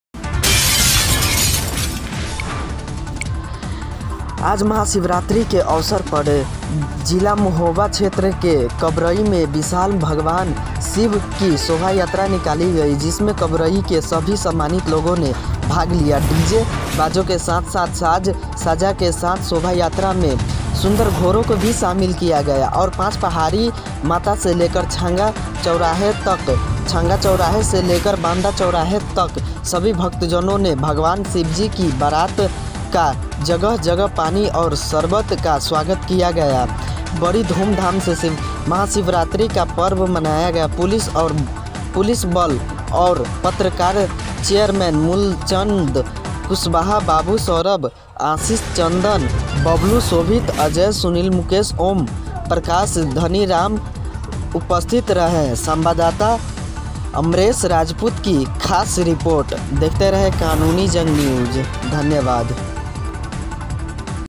आज महाशिवरात्रि के अवसर पर जिला महोबा क्षेत्र के कबरई में विशाल भगवान शिव की शोभायात्रा निकाली गई
आज महाशिवरात्रि के अवसर पर जिला महोबा क्षेत्र के कबरई में विशाल भगवान शिव की  शोभायात्रा निकाली गई जिसमें कबरई के सभी सम्मानित लोगों ने भाग लिया डीजे बाजों के साथ साथ साज सज्जा के साथ शोभायात्रा में सुंदर घोड़ो को भी शामिल किया गया और पांच पहाड़ी माता से लेकर छंगा चौराहे तक छंगा चौराहे से लेकर बांदा चौराहे तक सभी  भक्तजनों ने भगवान शिवजी की बारात का जगह जगह पानी और शरबत का स्वागत किया गया बड़ी धूमधाम से महाशिवरात्रि का पर्व मनाया गया। पुलिस बल और पत्रकार चेयरमैन मूलचंद कुशवाहा बाबू सौरभ आशीष चंदन बबलू शोभित अजय सुनील मुकेश ओम प्रकाश धनीराम उपस्थित रहे

अमरेश राजपूत की खास रिपोर्ट